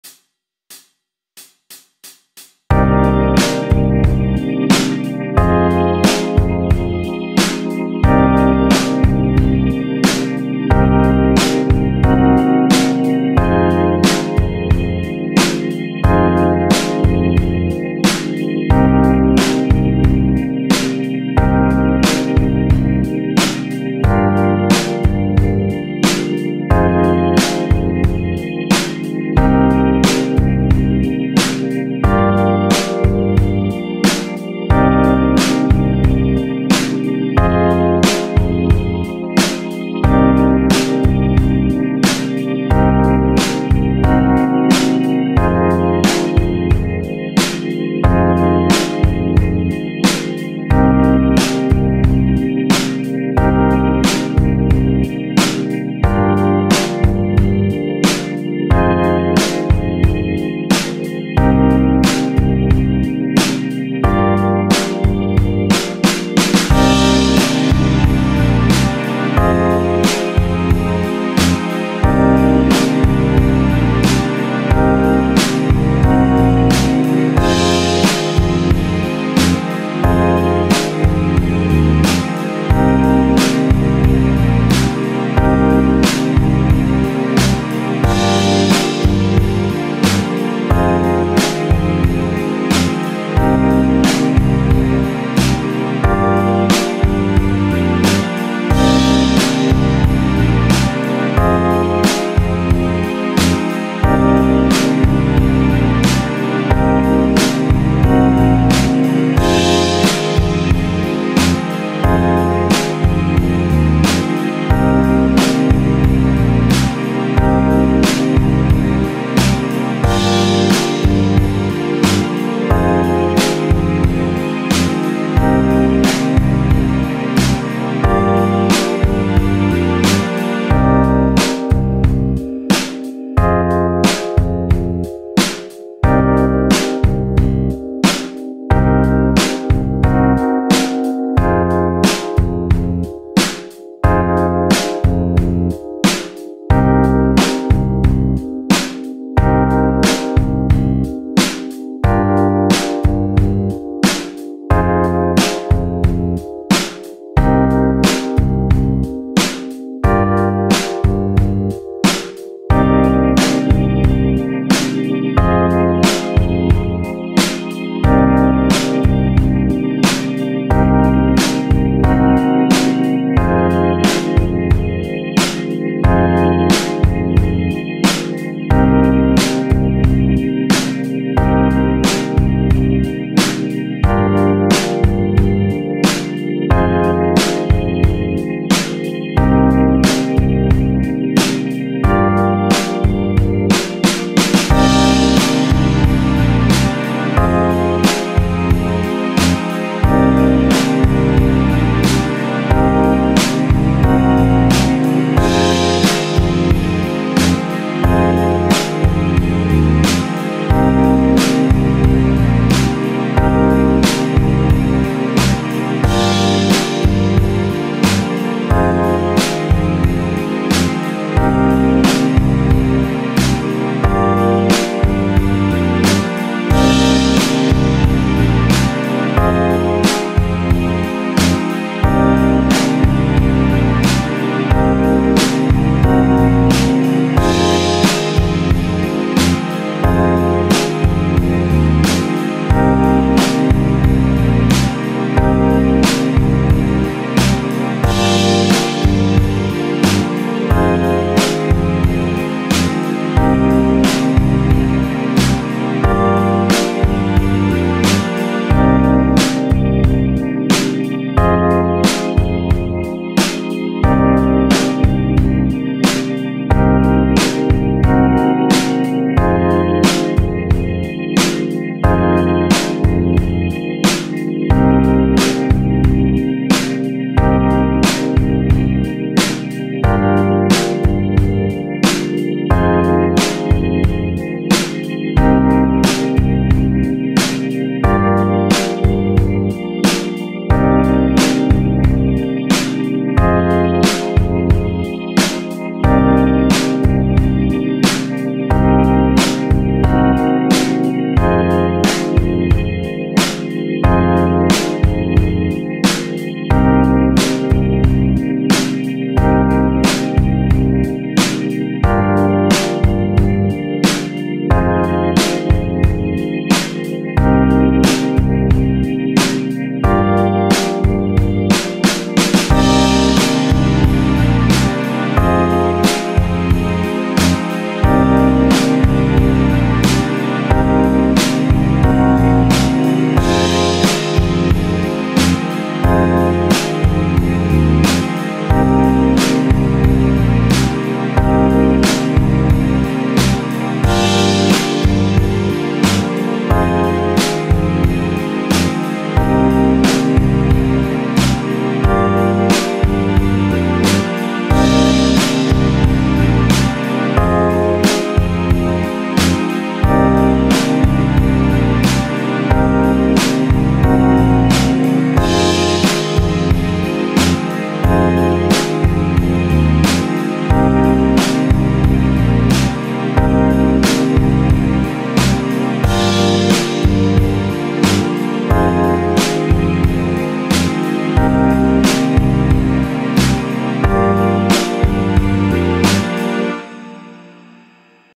Epic Blues Backing Track in Cm
Epic Blues Backing Track in Cm for lead or solo or improvisation practice.
Photo by Ash from Pexels